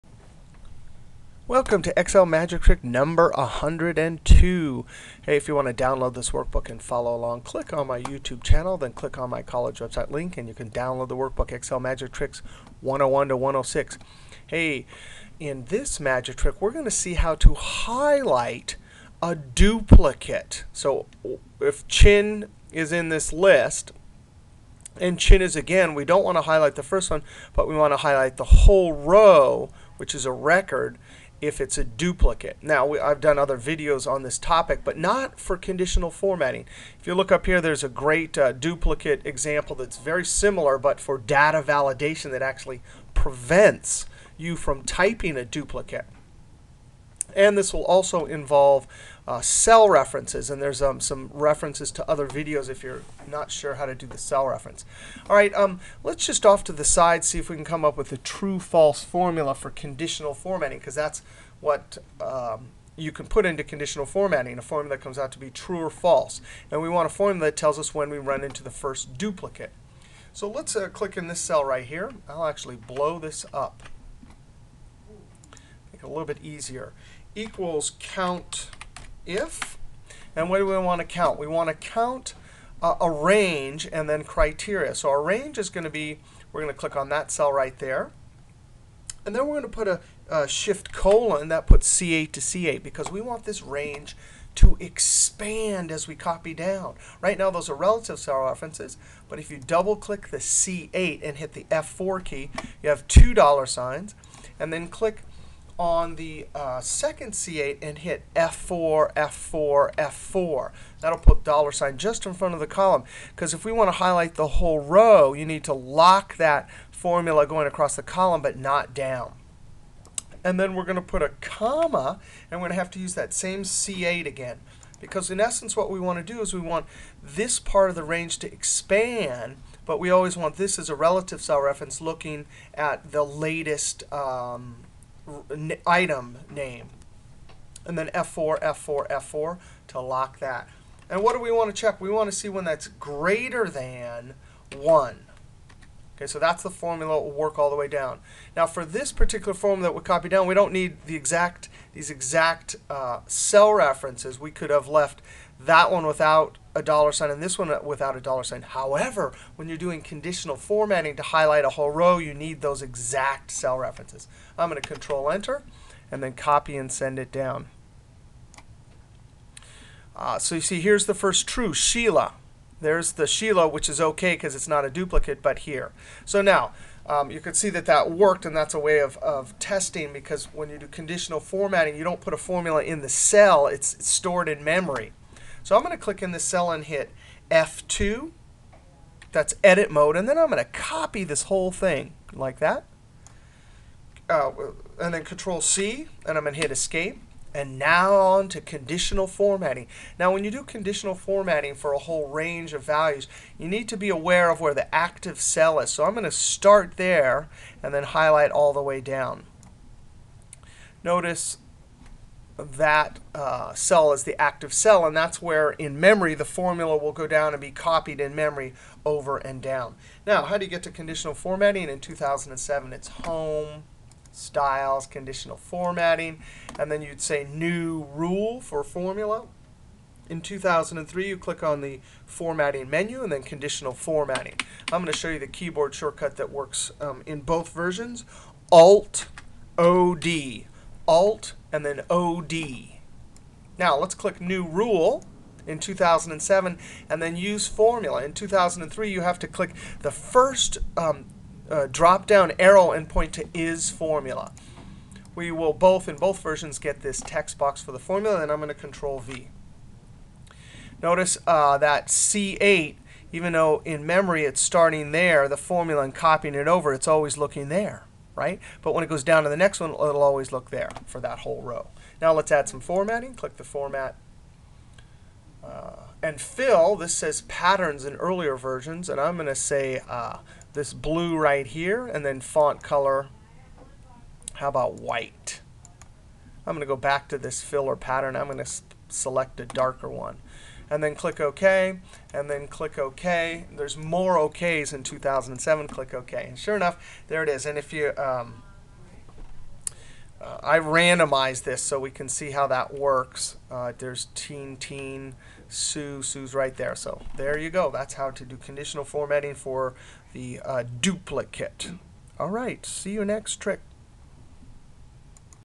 Excel Magic Trick #102: Conditional Formatting Duplicates
See how to add formatting to duplicate records using a TRUE FALSE COUNTIF formula in Conditional Formatting.